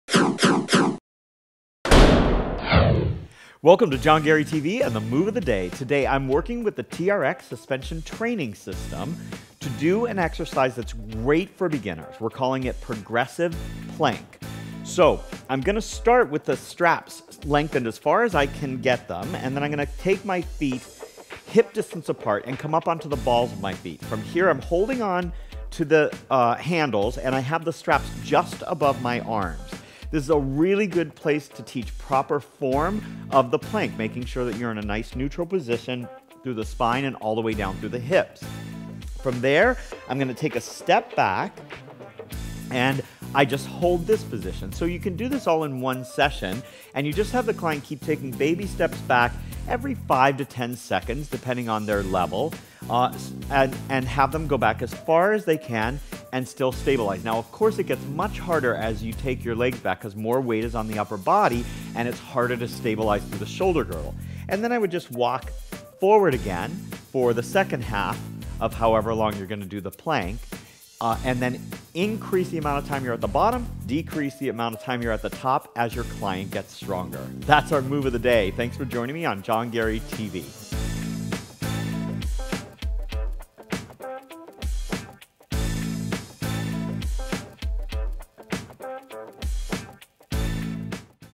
Plank Progression with TRX
JohnGareyTV.com presents the Move of the Day: Progressive Plank with the TRX.  This is a great way to introduce the plank to new clients and to add variety to the regular plank for all clients.  Adjust the time and the foot position to make it more or less challenging.